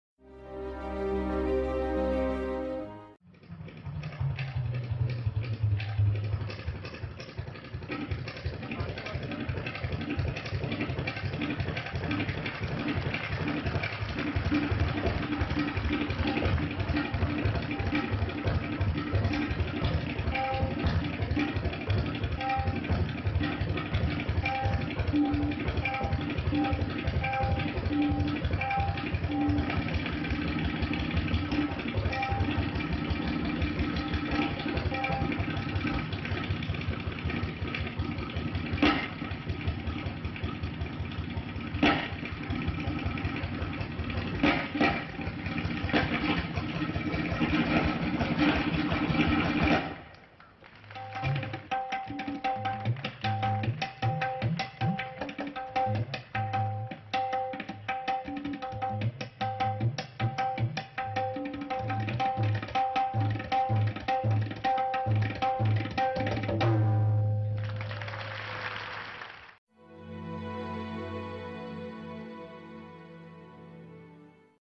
Tabla Maestro Zakir Hussain Interacts With Yerawada Prison Inmates - Patrika News
Tabla maestro Zakir Hussain gave a special treat to inmates at Pune's Yerawada Central Prison with his performance on Friday. As a part of the rehabilitation program 'Preranapath', tabla maestro Zakir Hussain visited the jail and interacted with the inmates. The event was organised by a NGO and the prison administration.